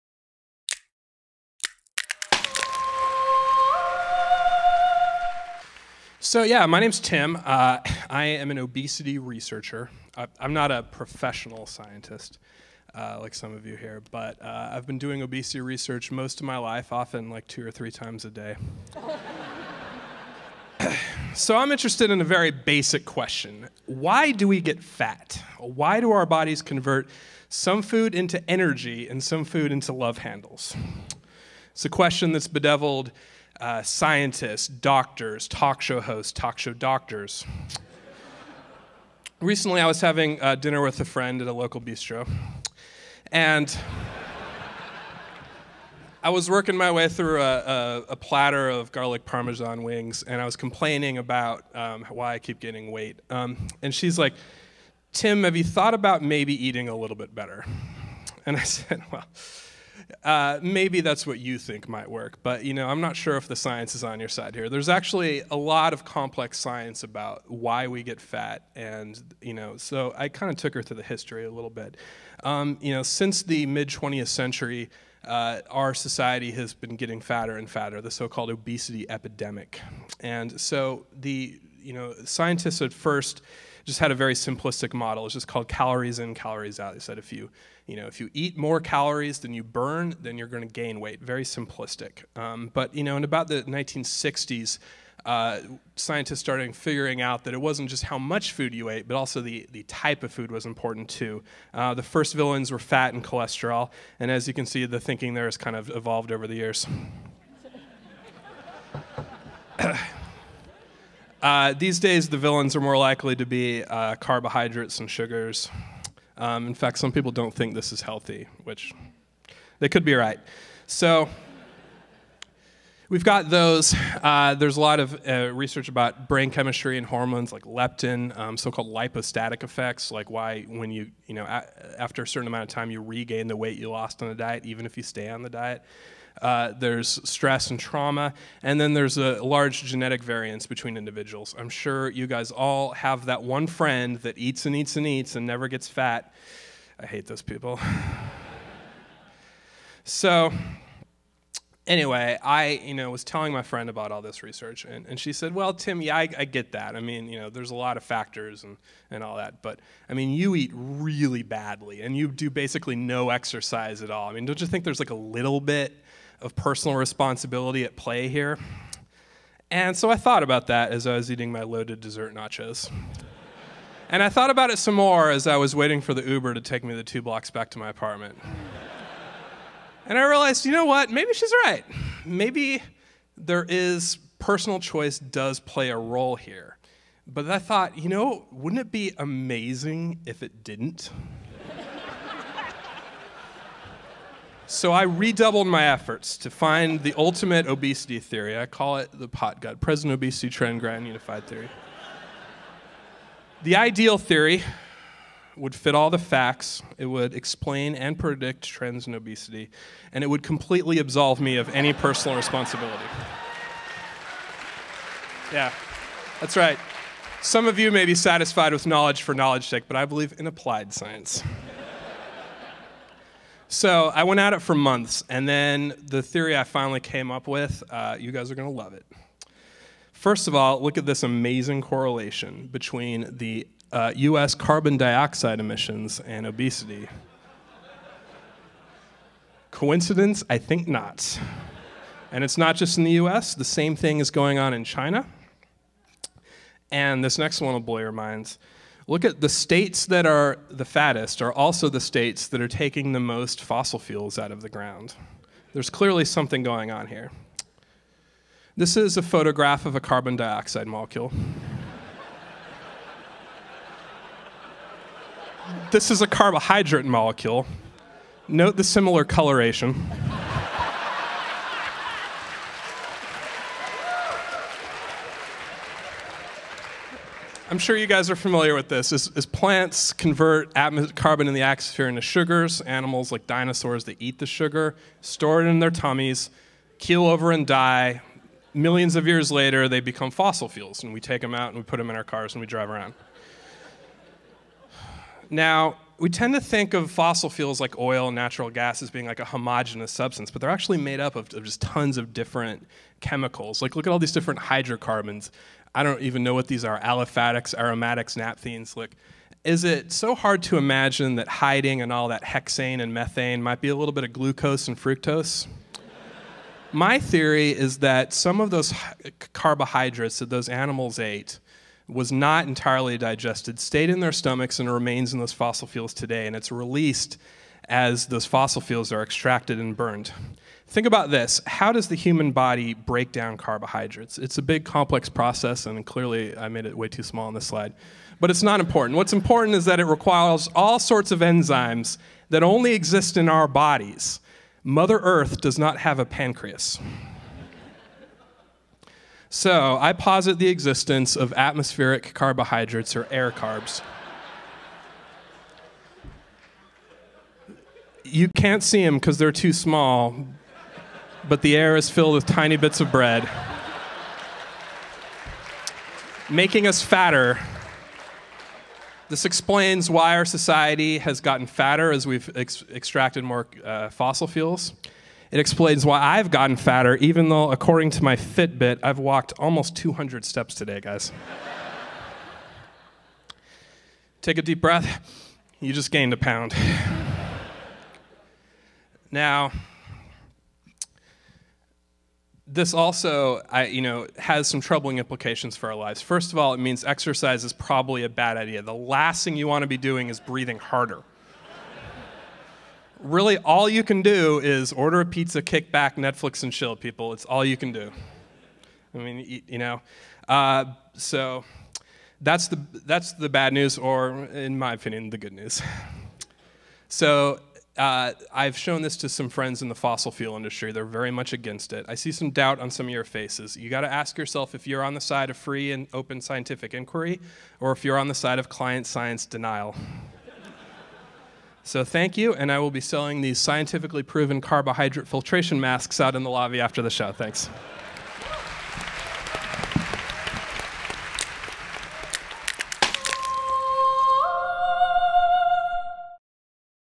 BAHFest Seattle 2017 - Tim Crockett: Food and Air: Climate Change and the Global Obesity Epidemic
Tim Crockett discusses his theory of the connection between the increase of CO2 emissions and the rise in obesity.

BAHFest is the Festival of Bad Ad Hoc Hypotheses, a celebration of well-researched, logically explained, and clearly wrong scientific theory.